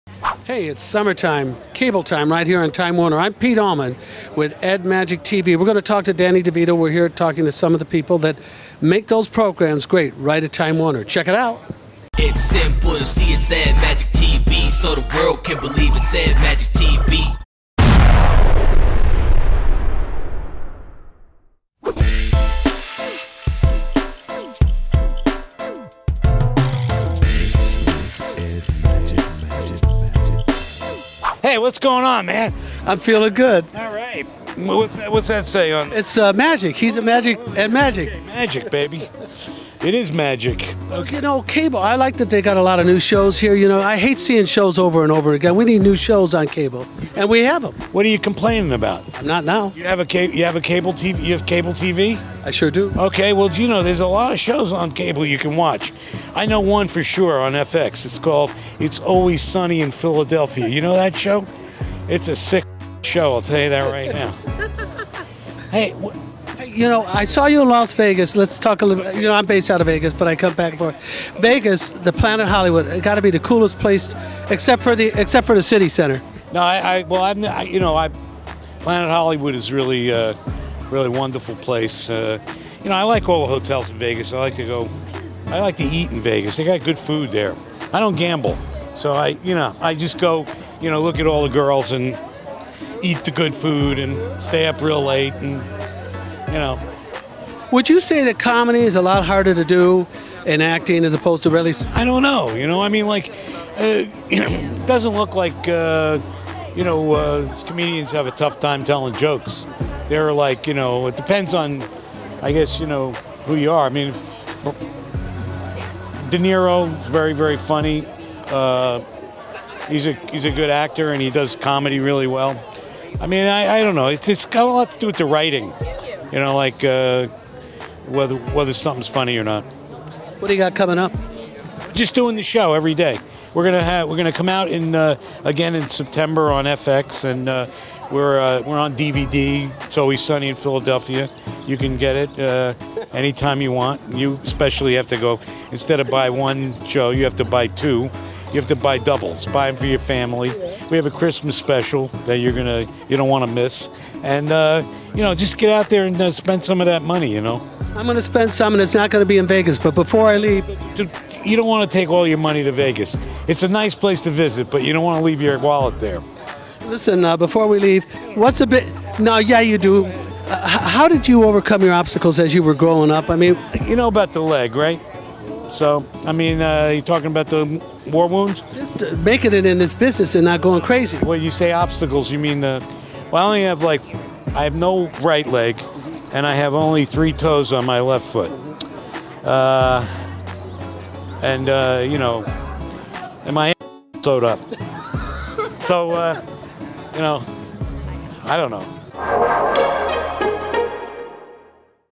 Danny DeVito Interview on the Blue Carpet of Time Warner Cable's Summer Time Event
Danny DeVito Interview on the Blue Carpet of Time Warner Cable's Summer Time Event .  This video is part of a series playlist for entertainment updates.